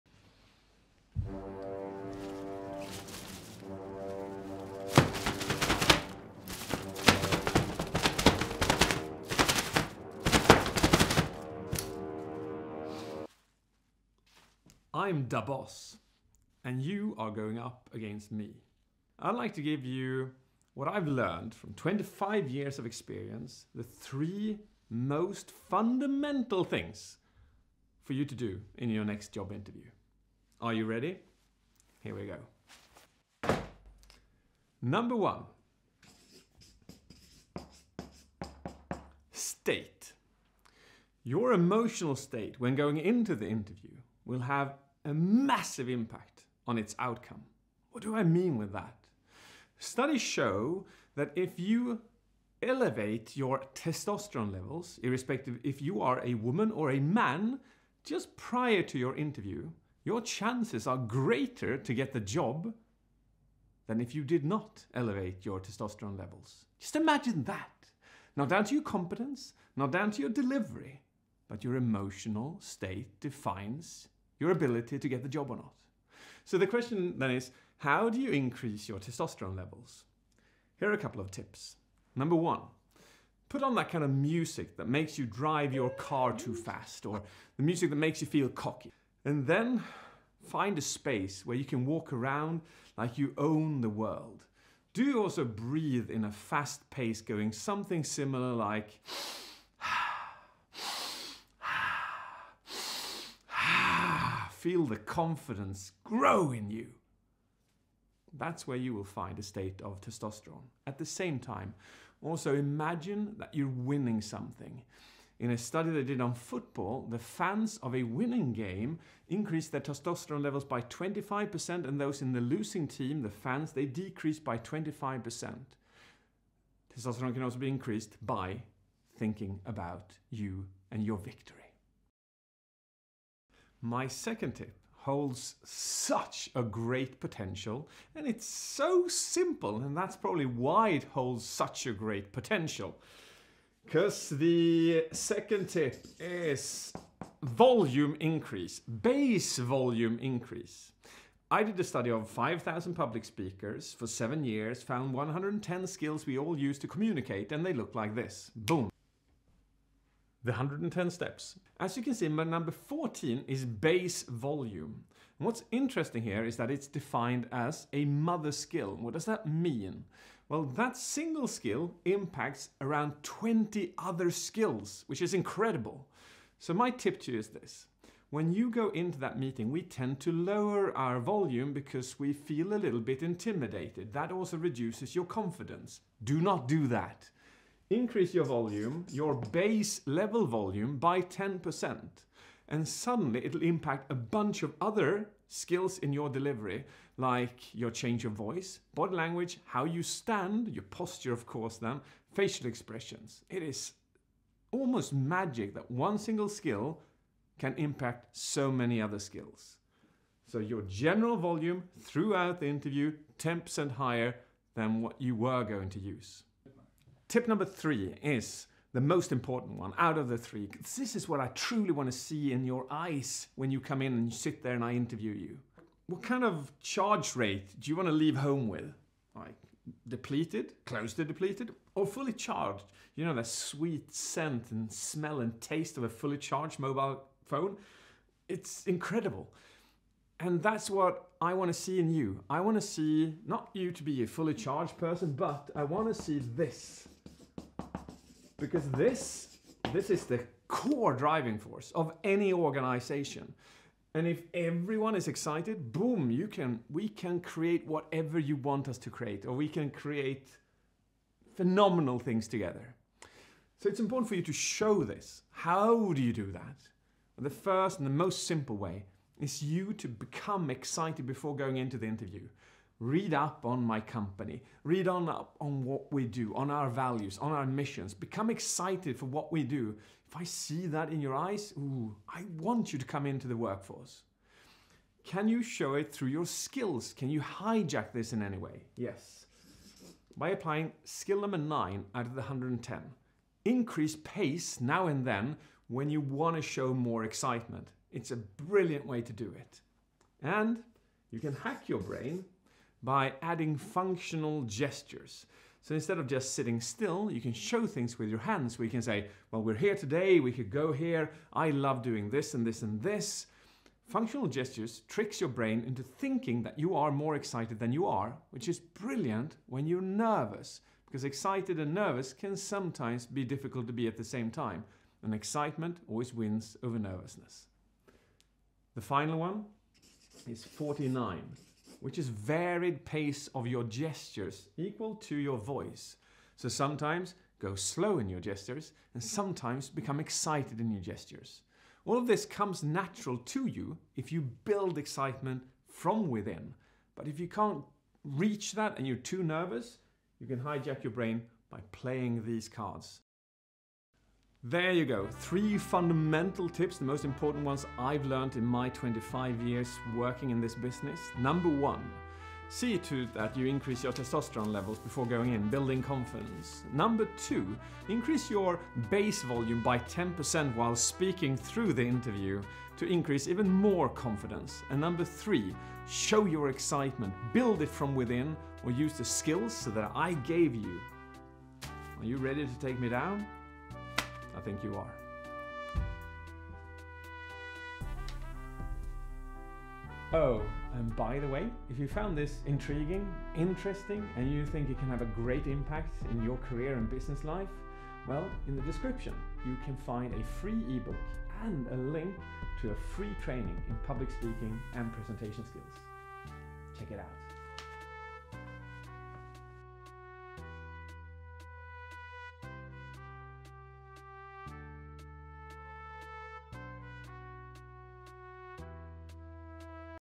How to Do Well in a Job Interview - 3 Tips To Help You Succeed
How do you do well in a job interview? These are three quick communication and presentation tips to help you with business and be professional in a work setting!